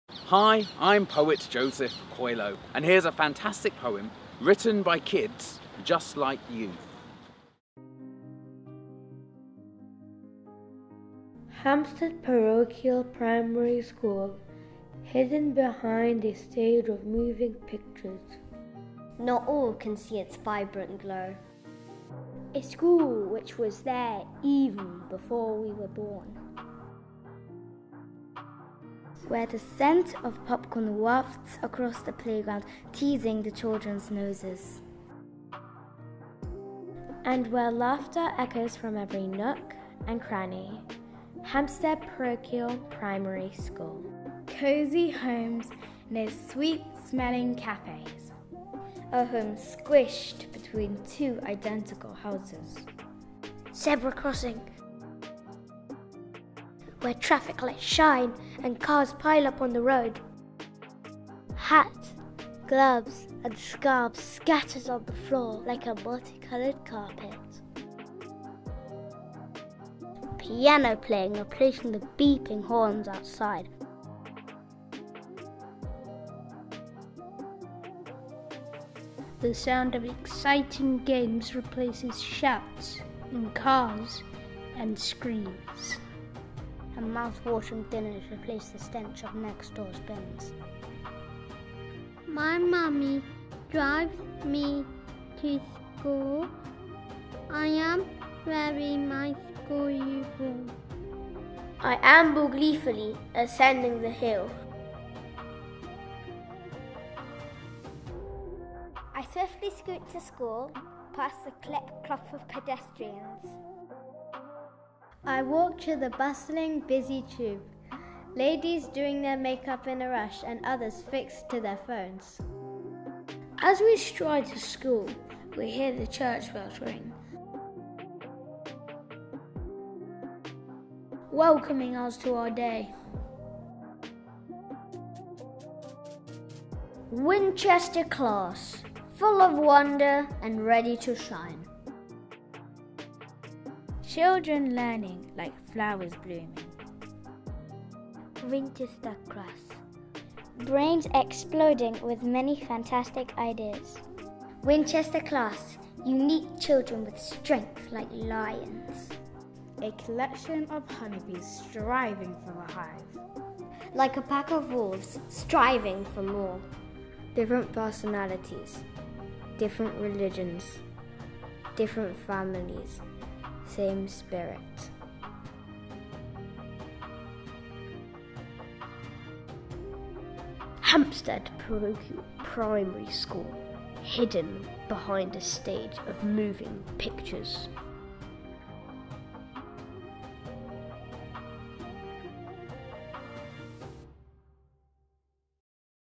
This Is Us Poetry | Hampstead Parochial | Joseph Coelho
The children at Hampstead Parochial Primary in Hampstead, London, share their life experiences and culture through their poetry.

The poetry writing unit 'This Is Us!' allows children to tell their stories with the help of acclaimed poet Joseph Coelho.